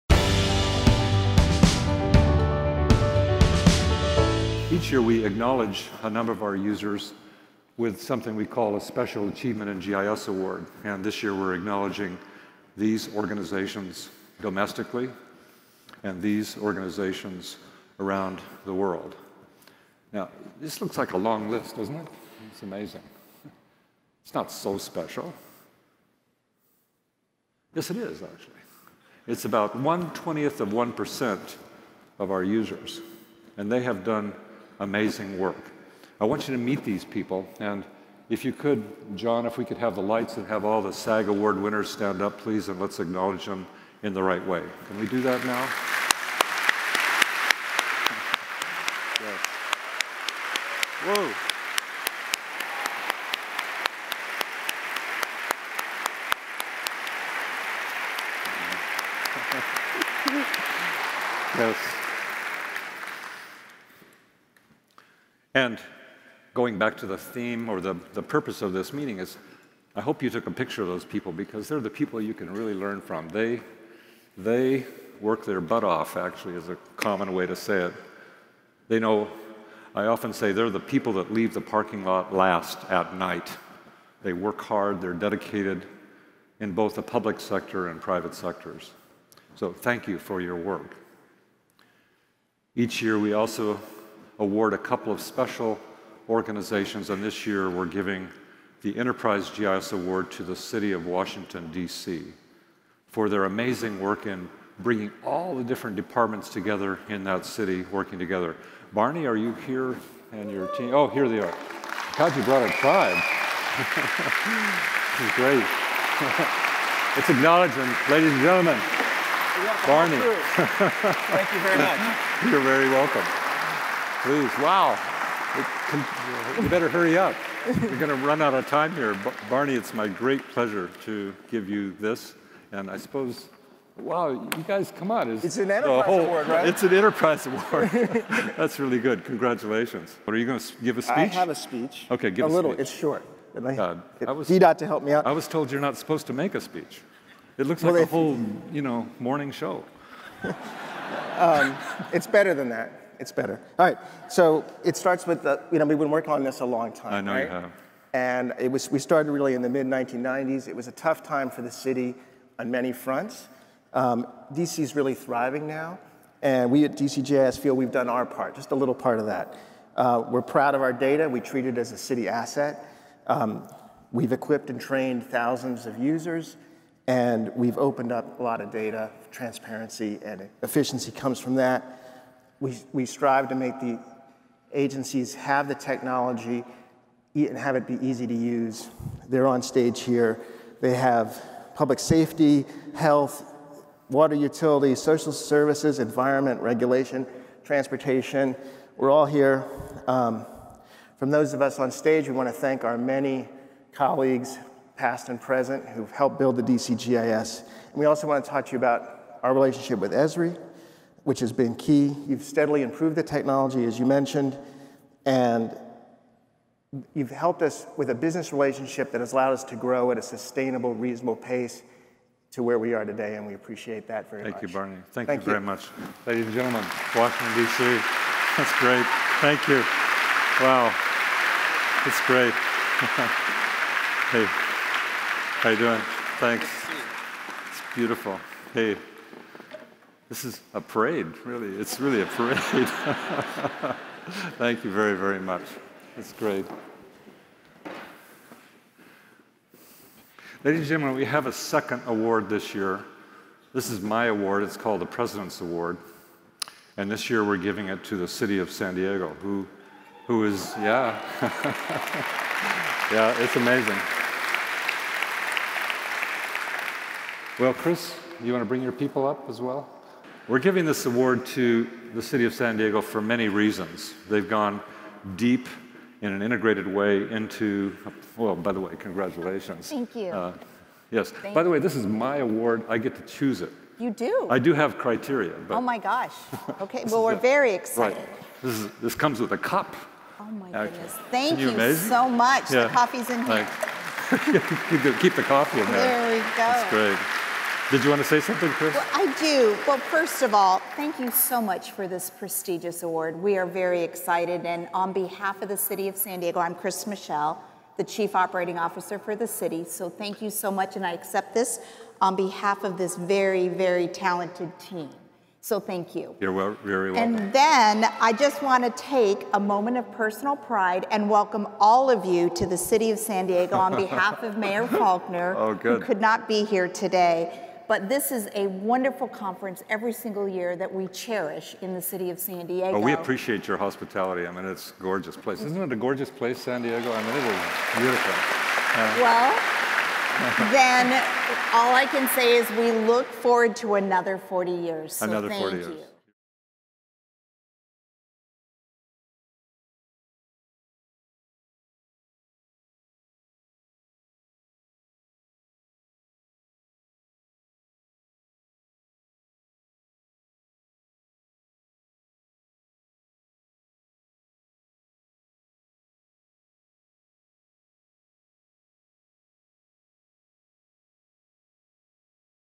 An Award-Winning Community
Esri users are an award-winning community. See Jack Dangermond introduce hundreds of inspiring maps, and watch this year's award winners, including the Special Achievement in GIS award, President's Award, and Enterprise GIS Award. See these innovative organizations accept awards on the UC stage.